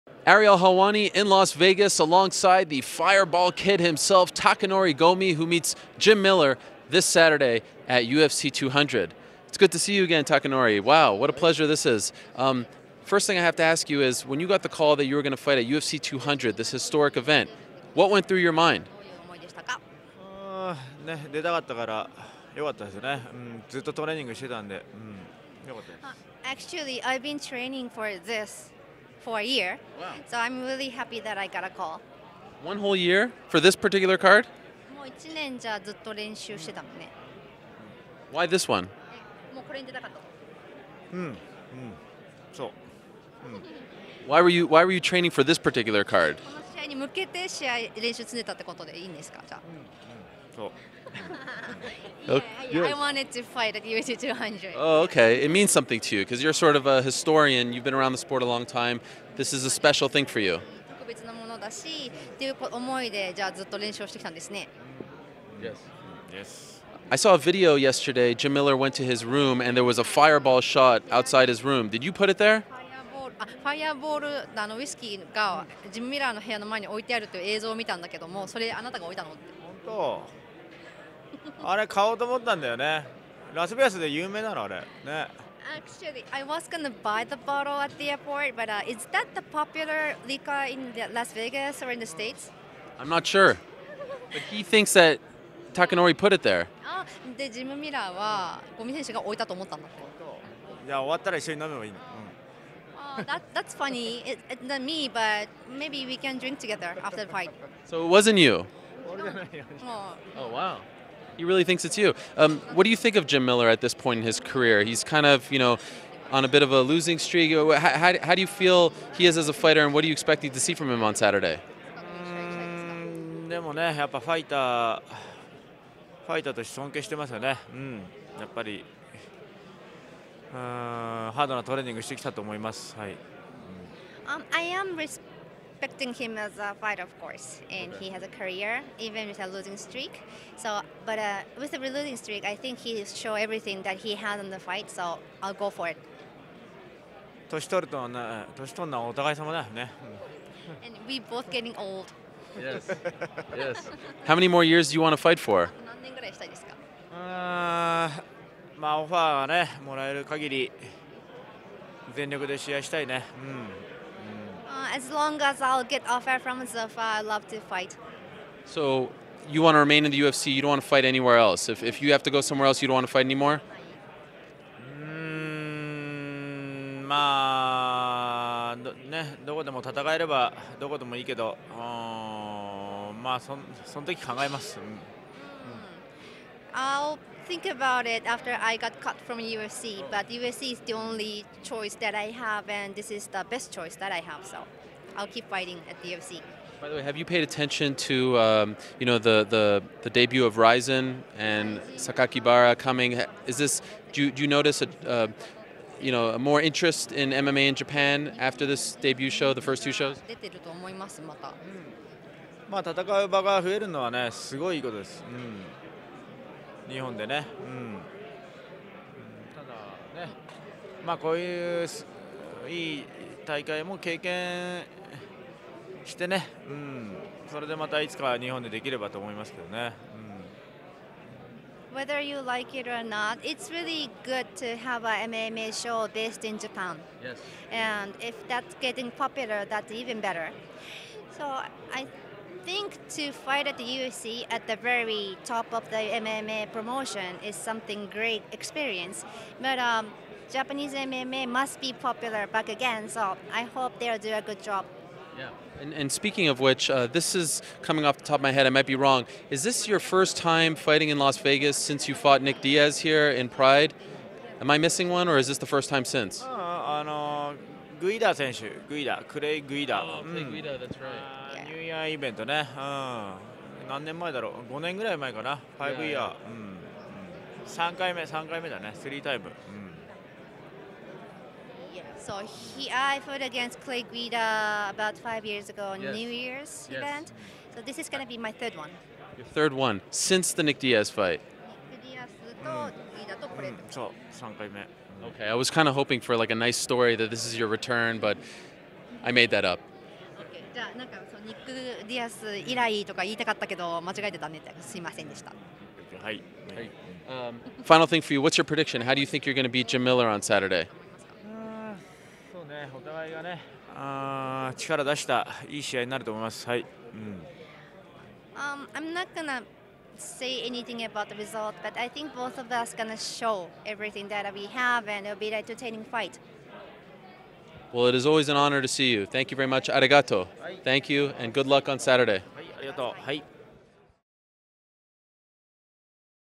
UFC 200: Takanori Gomi Began Training for UFC 200 a Year Ago
Takanori Gomi talks about his UFC 200 fight against Jim Miller, why he wanted to fight on this card, the state of Japanese MMA and more.